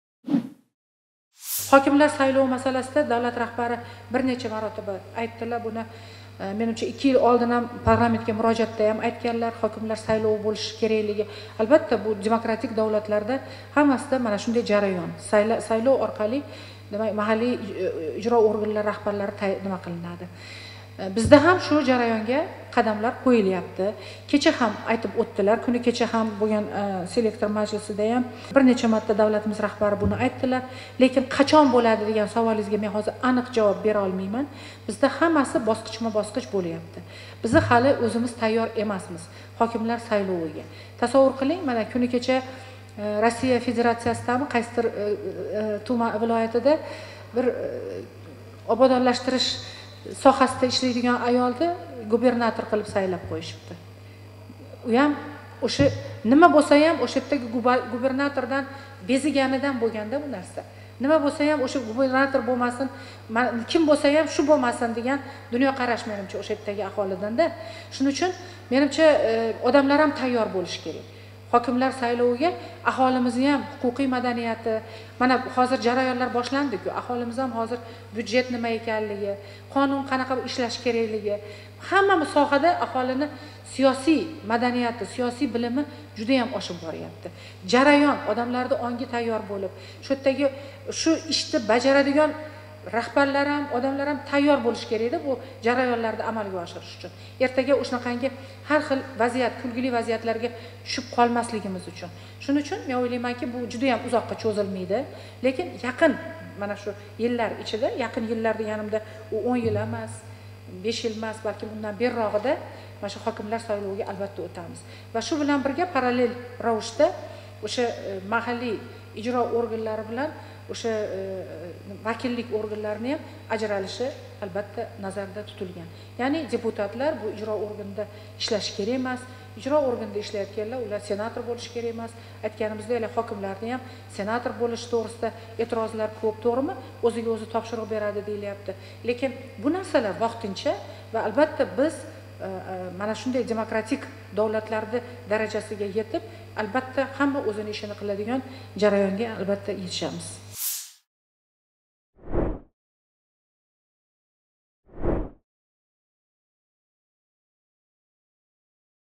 «Yaqin yillar ichida hokimlarni saylash tizimiga o‘tamiz» – Tanzila Norboyeva
Oliy Majlis Senati raisi Tanzila Norboyeva hokimlar albatta saylanishi kerakligi, biroq bu jarayon bosqichma-bosqich bir necha yil davomida amalga oshirilishini aytib o‘tdi. U bu haqda bugun – 6-oktyabr kuni Toshkent viloyati hokimligida bo‘lib o‘tgan Senati kengashining sayyor majlisidan so‘ng tashkil etilgan matbuot anjumanida Kun.uz muxbirining savoliga javoban maʼlum qilgan.

Senat raisi hokimlarning senator bo‘lishi borasida jamoatchilikda eʼtirozlar ko‘p ekani xususida ham to‘xtalib, mahalliy ijro organlari bilan vakillik organlarini ham ajralishi kerakligini taʼkidlagan. Uning aytishicha, deputatlar ijro organida ishlamasligi, ijro organida ishlaydiganlar senator bo‘lmasligi kerak.

KUN.UZ yangiliklar saytining Youtube`dagi rasmiy kanaliga xush kelibsiz!